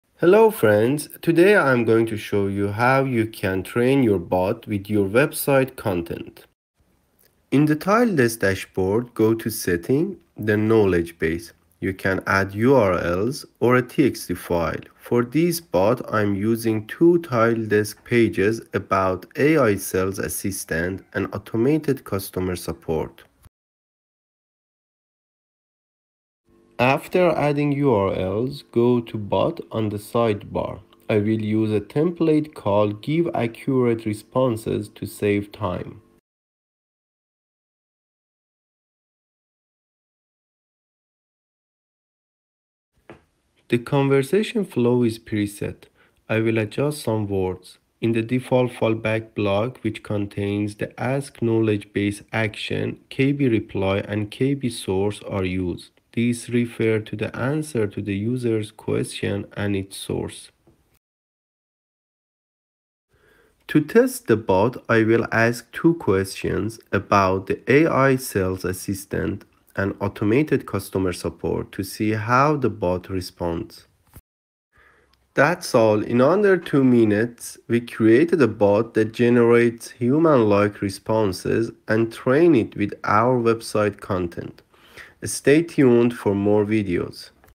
New Knowledge Base System - Training Your Chatbot with Website Content
Discover Tiledesk’s latest feature in our knowledge base system. This groundbreaking update allows you to add your website URLs for indexing. In this tutorial, I’ll guide you through accessing the ‘Knowledge Base’ and demonstrate how to add your URLs for indexing. We’ll also delve into chatbot creation using a ready-to-use chatbot template, showcasing the simplicity of getting started with a chatbot template. These templates are highly customizable, allowing you to tailor every aspect to suit your needs. I’ll also explain the workings of the ChatGPT action and how it empowers your chatbot to generate human-like responses. Be sure to watch the video to the end where we’ll put our chatbot to the test together.

We have more exciting videos coming your way, so keep an eye out for them!